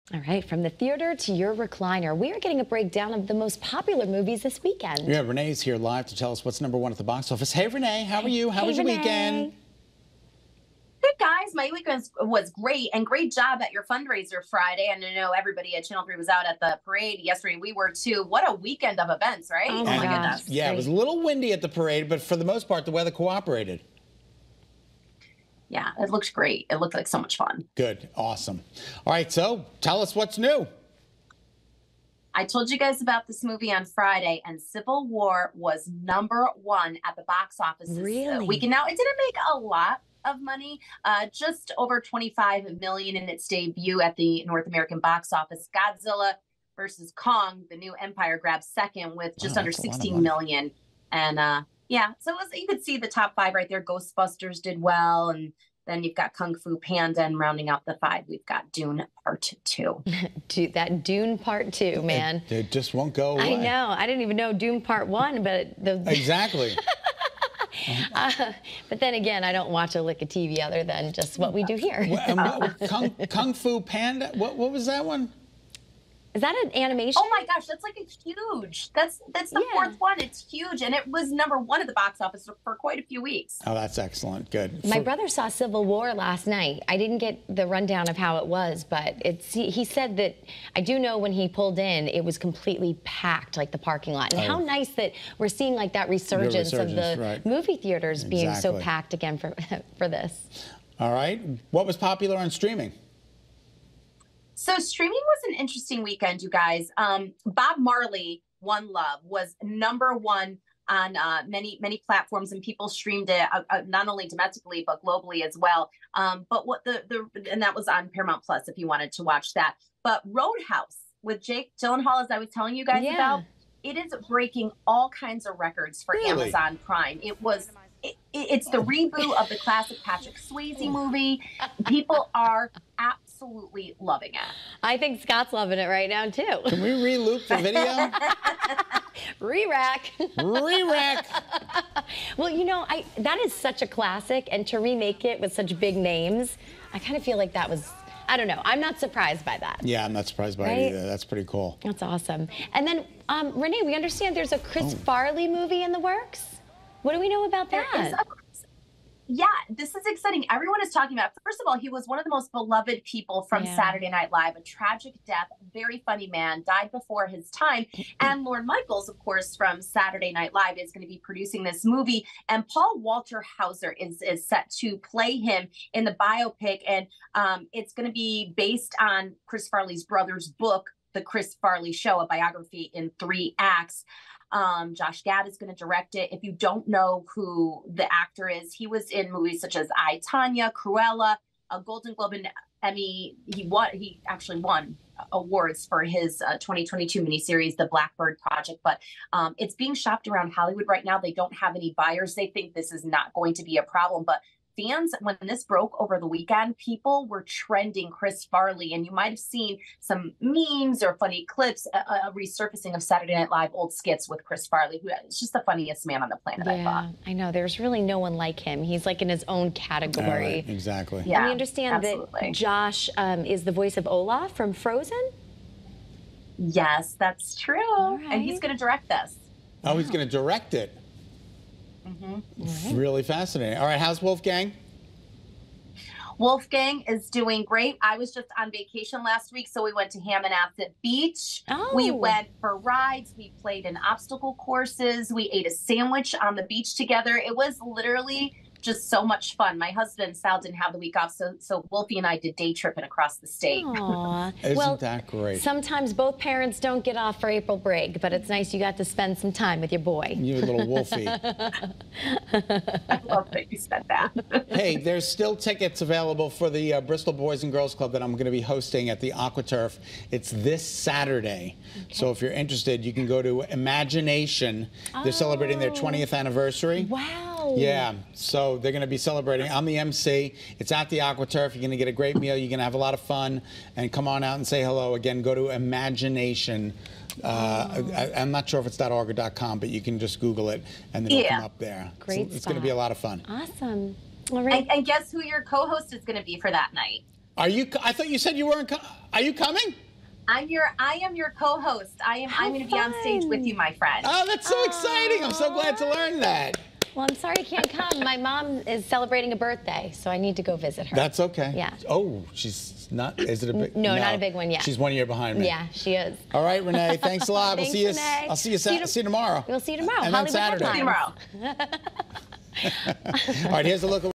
Weekend Box Office
Renee DiNino is here to tell us what was popular at the box office this weekend!